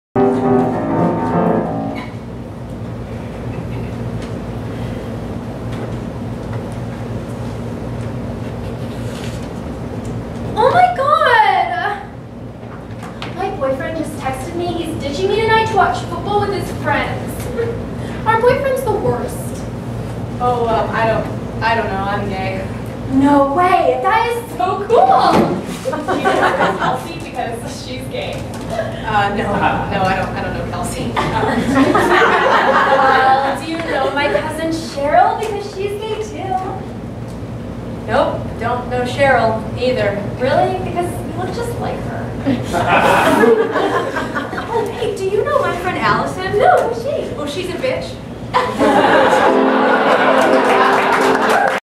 Allison
From Quixotic's Summer 2011 production of "Civil Rights & Left Overs" at Donny's Skybox. Written by Robin Trevino. Performed by Jaclyn Keough and Kay. Directed by Dan Findley.